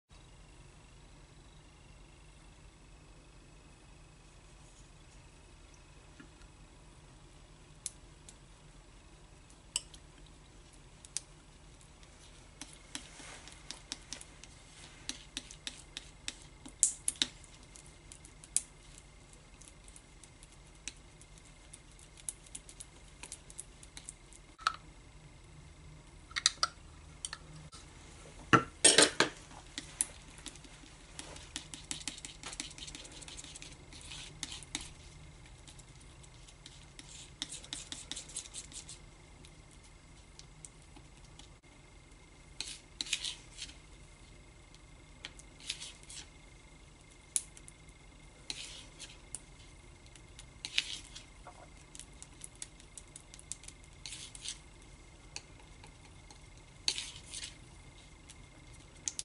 How DO you restore dried out Ultramarine Blue Oil Paint! 🤔
Early morning in the studio... Preparing for the days painting and restoring some ANCIENT and super crusty ultramarine blue - I only just managed to squeeze this out of the tube! Buttery consistency?... erm... nope!!